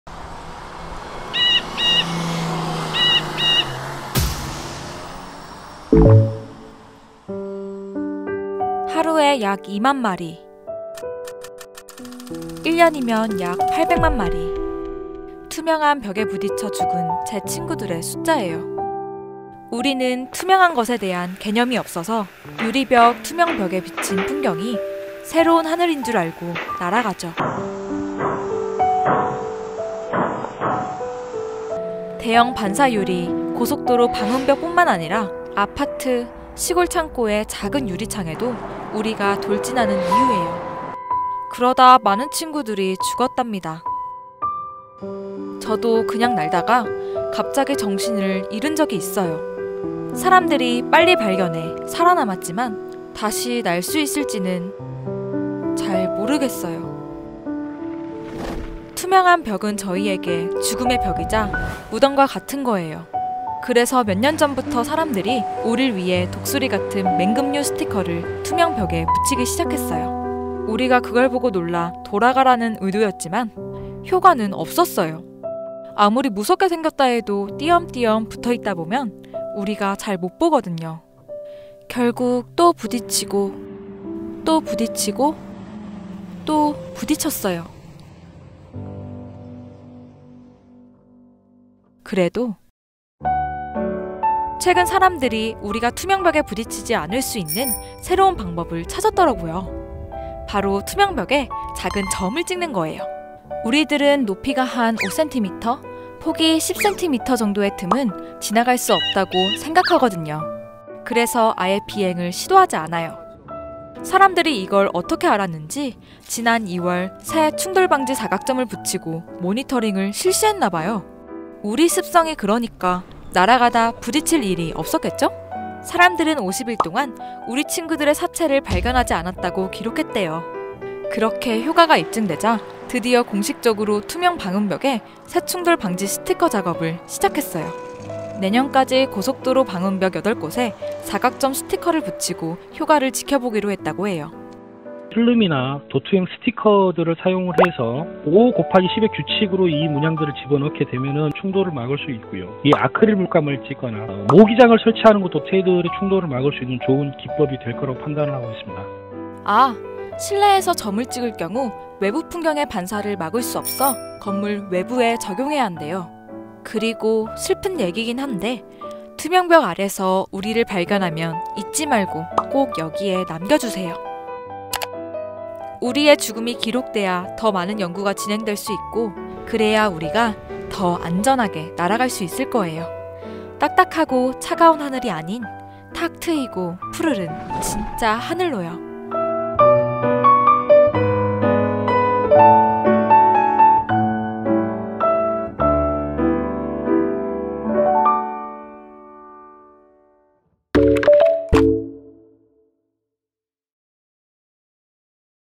투명 방음벽, 유리창에 부딪혀 죽는 800만 마리의 새를 살리는 방법 / 스브스뉴스
건너편이 훤히 들여다보이는 고속도로 방음벽, 파란 하늘을 거울처럼 비추는 건축물의 반사 유리. 매년 약 800만 마리의 새가 이런 '투명한 벽'에 부딪혀 죽어가고 있습니다. 새들의 불필요한 죽음을 막기 위해 지난 2007년부터 '버드세이버'라고 불리는 맹금류 스티커가 널리 쓰이기 시작했지만, 이는 효과가 없는 것으로 밝혀졌습니다.
하지만 다행히도, 새들에게 투명창의 존재를 인식시킬 또 다른 방법들이 있다고 합니다. 바로 유리창에 5x10cm 이내 간격으로 작은 점들을 찍어주는 것. 고속도로 방음벽뿐만 아니라 일상생활에서도 적용할 수 있는 방법입니다.
이 외에도 다양한 새 충돌 저감 대책들을 스브스뉴스가 알아보았습니다.